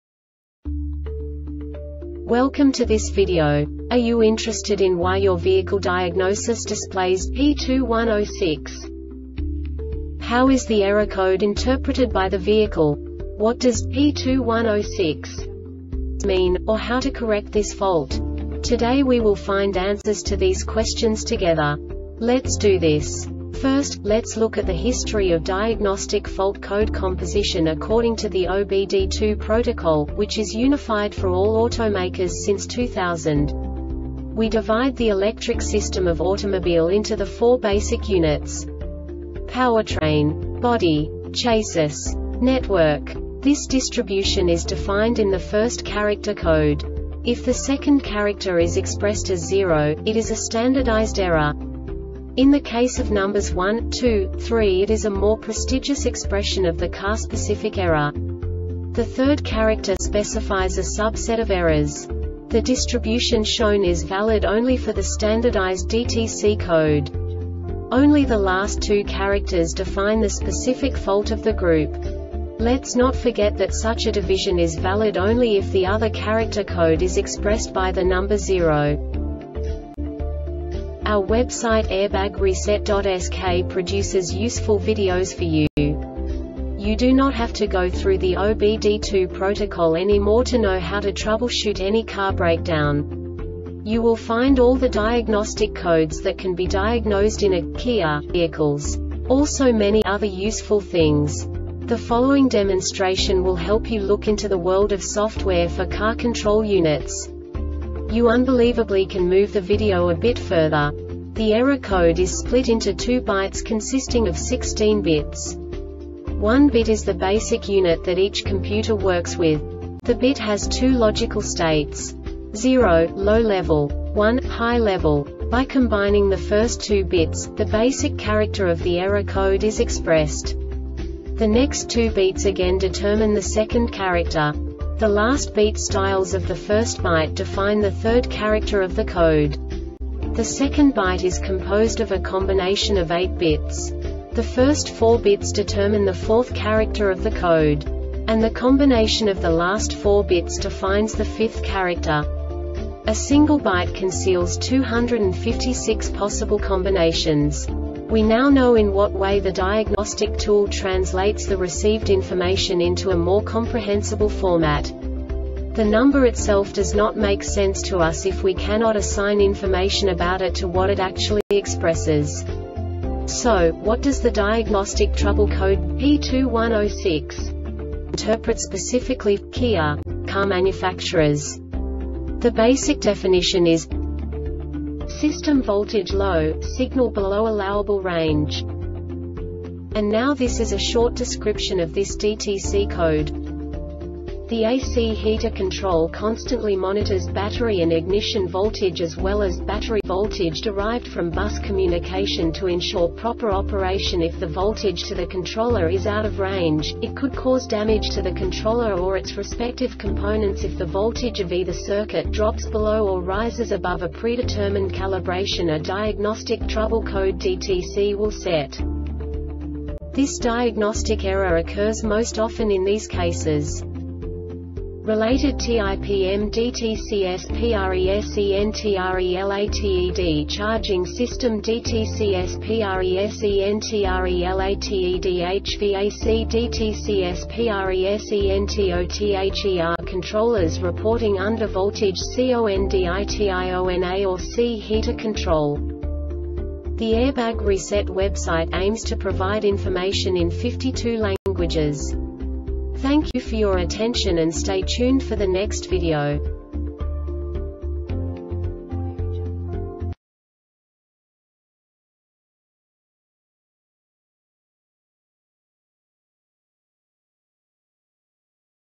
DTC KIA P2106 Short Explanation
Contents:
0:21 Basic DTC analysis according to OBD2 protocol standard.
1:48 Insight into programming
2:58 A diagnostically understandable description of the Diagnostic Trouble Code
3:05 Basic error definition

Make:
Kia
Code:
P2106
Definition:
System Voltage Low - Signal Below Allowable Range
Description:
The A or C Heater Control constantly monitors battery and ignition voltage as well as battery voltage derived from BUS communication to ensure proper operation. If the voltage to the controller is out of range, it could cause damage to the controller or its respective components. If the voltage of either circuit drops below or rises above a predetermined calibration a Diagnostic Trouble Code (DTC) will set.
Cause:
RELATED TIPM DTCS PRESENTRELATED CHARGING SYSTEM DTCS PRESENTRELATED HVAC DTCS PRESENTOTHER CONTROLLERS REPORTING UNDER VOLTAGE CONDITIONA or C HEATER CONTROL